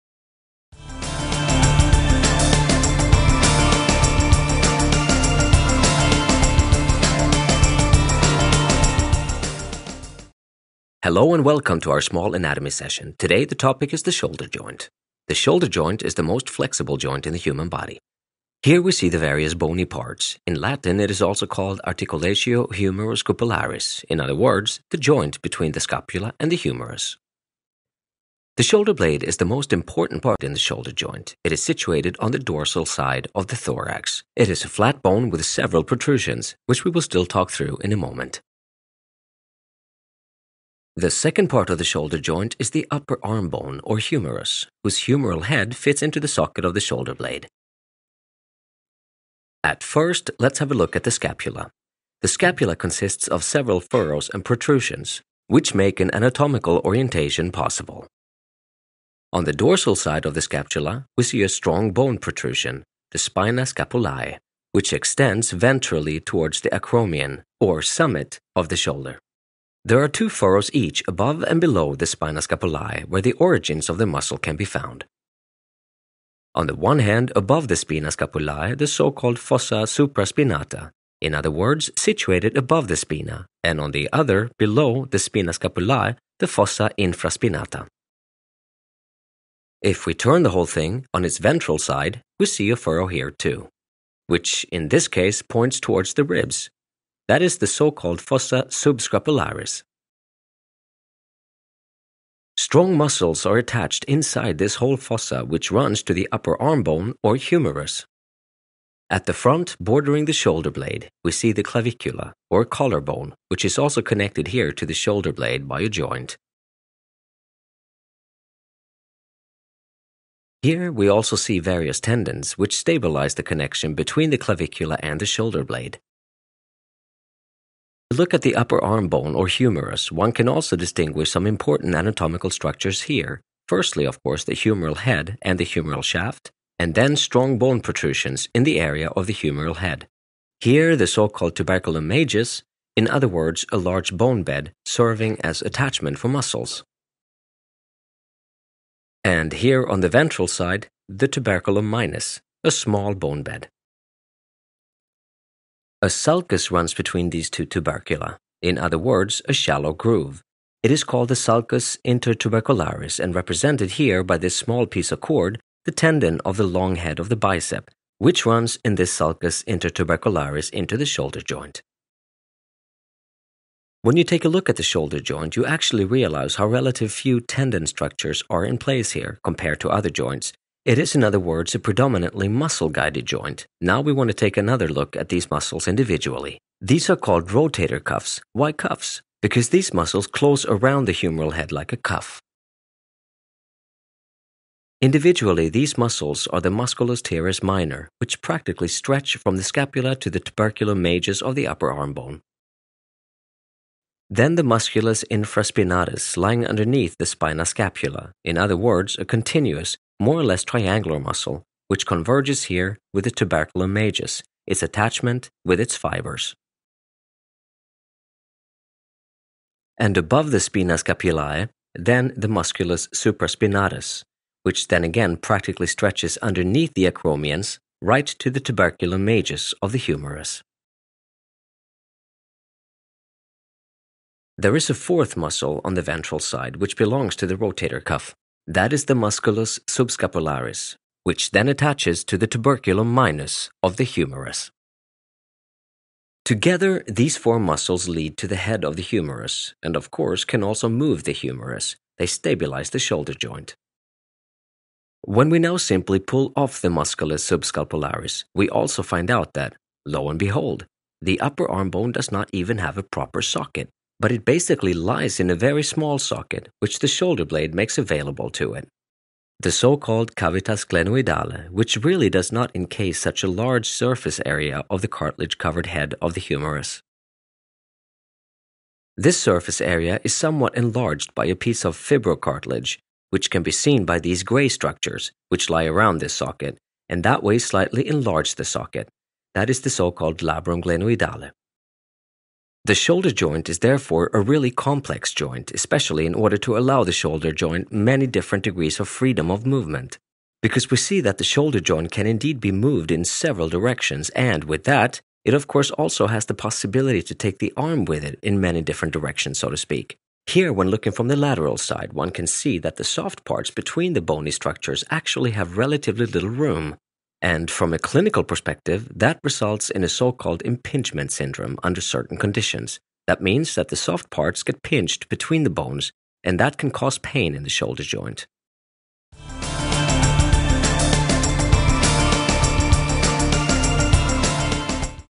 Anatomy: The Shoulder Joint
The 3B Scientific® Anatomy Video "Shoulder Joint" vividly describes the functional and topographical anatomy of the shoulder joint. In addition to naming each of the bony structures and muscles of the rotator cuff, all the natural movements of the shoulder joint are shown in detail.